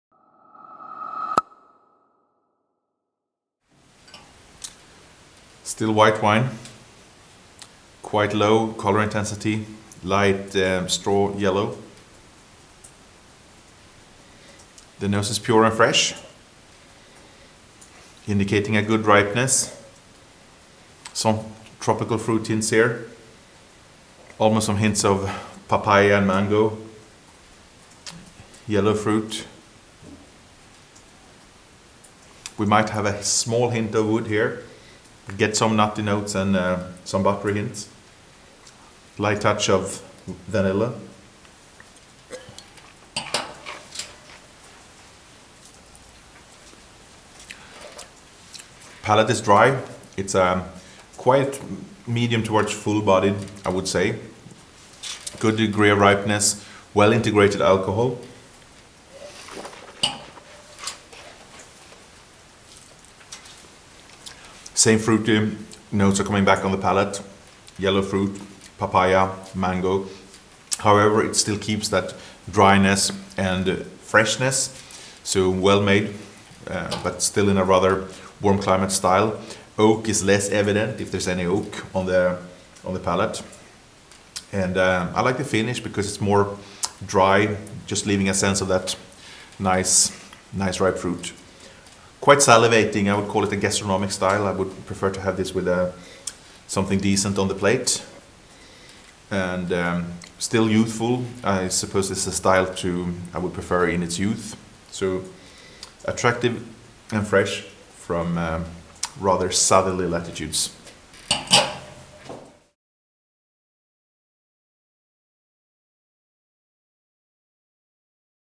Domaine De Baron'arques - Le Chardonnay 2013 - Limoux
Still white wine, quite low colour intensity, light straw yellow. The nose is pure and fresh, indicating a good ripeness. Some tropical fruits hints here, almost some hints of papaya and mango, yellow fruit. We might have a small hint of wood here; I get some nutty notes and some buttery hints,  light touch of vanilla. The palate is dry, it’s quite medium towards full-bodied. Good degree of ripeness, well-integrated alcohol. Same fruity notes are coming back on the palate, yellow fruit, papaya, mango; however it still keeps that dryness and freshness. So well-made, but still in a rather warm climate style. Oak is less evident, if there’s any oak on the palate. I like the finish, because it’s more dry, just leaving a sense of that nice ripe fruit. Quite salivating, I would call it a gastronomic style, I would prefer to have this with something decent on the plate. Still youthful, it’s a style that I’d prefer in its youth. Attractive and fresh, from rather southerly latitudes.